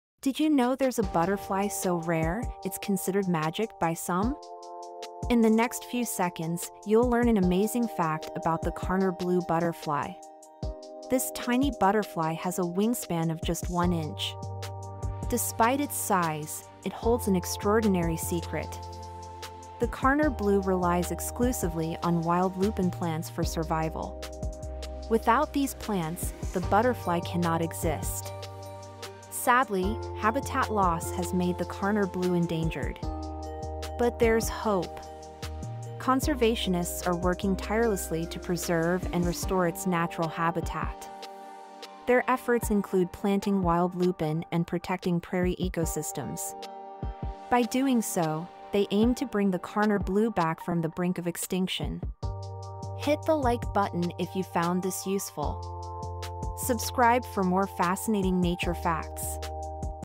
I Found a Real Life Magical Butterfly!
Did you know there's a butterfly so rare, it's considered magic by some?
In the next few seconds, you'll learn an amazing fact about the Karner Blue butterfly.
This tiny butterfly has a wingspan of just one inch.
Despite its size, it holds an extraordinary secret.
The Karner Blue relies exclusively on wild lupine plants for survival.
Without these plants, the butterfly cannot exist.
Sadly, habitat loss has made the Karner Blue endangered.
But there’s hope.
Conservationists are working tirelessly to preserve and restore its natural habitat.
Their efforts include planting wild lupine and protecting prairie ecosystems.
By doing so, they aim to bring the Karner Blue back from the brink of extinction.